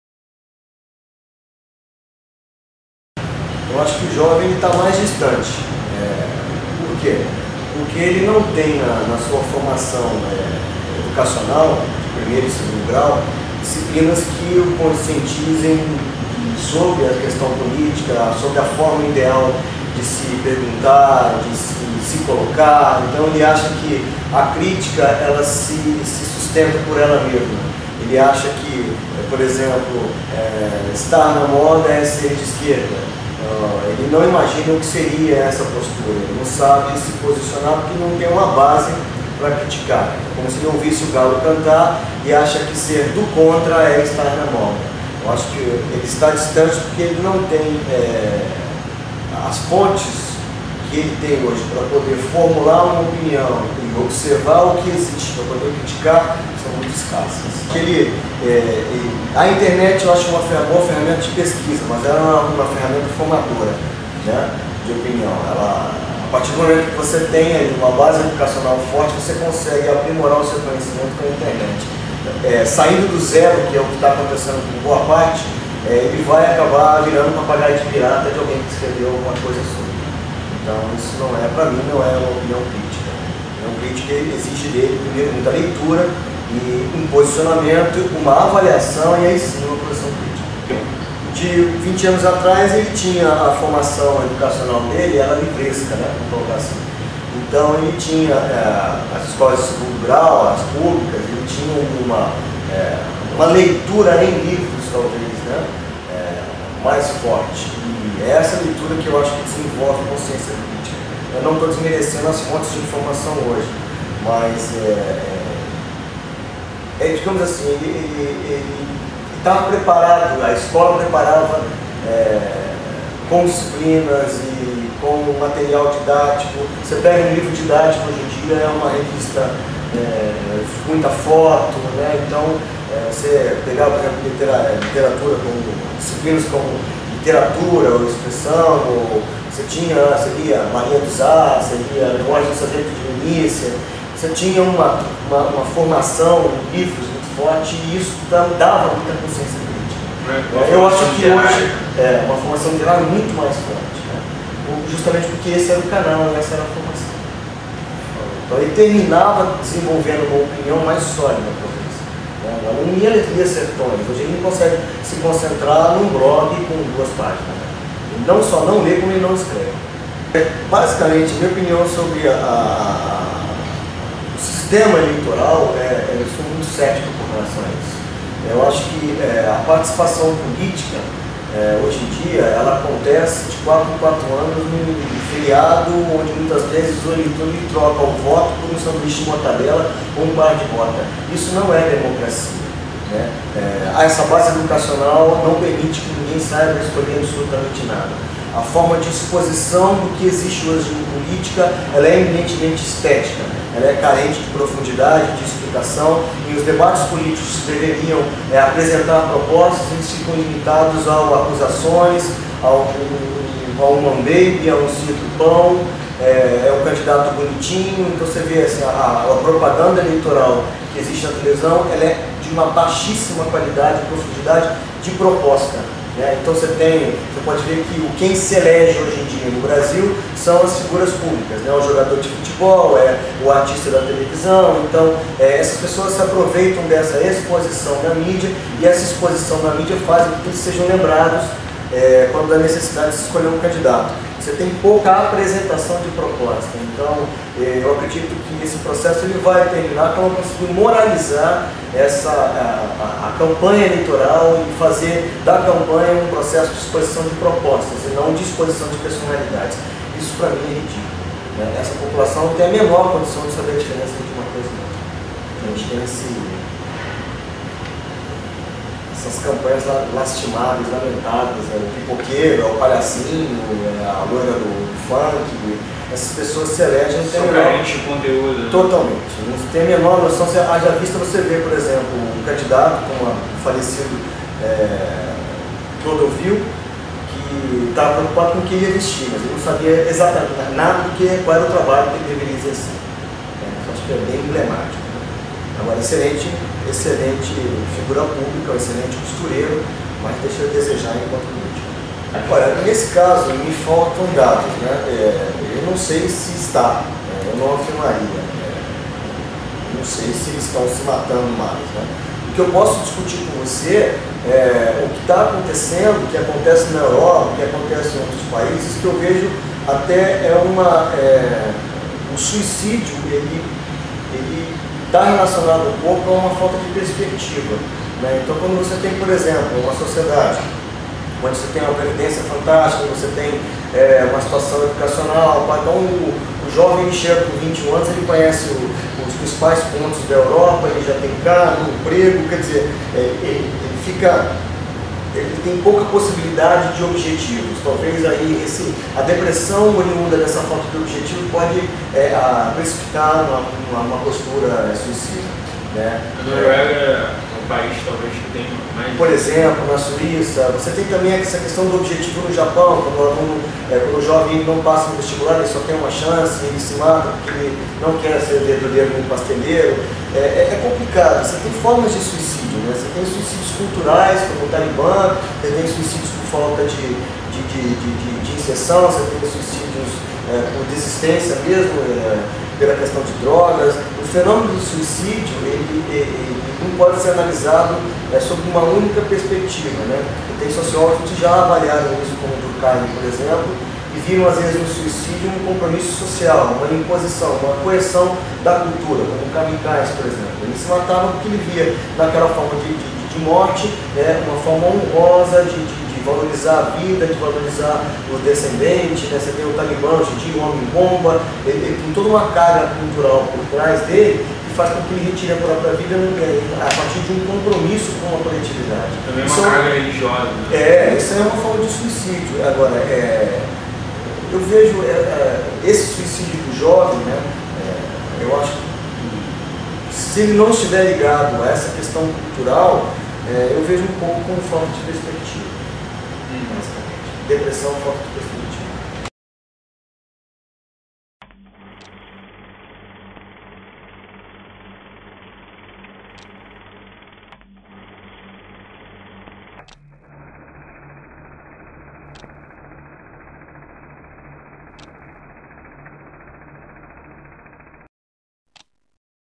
Escancarando com Marco Antônio Moreira - Parte 1
Marco Antônio Moreira, Professor de Sociologia da ESPM-RJ, escancara temas polêmicos.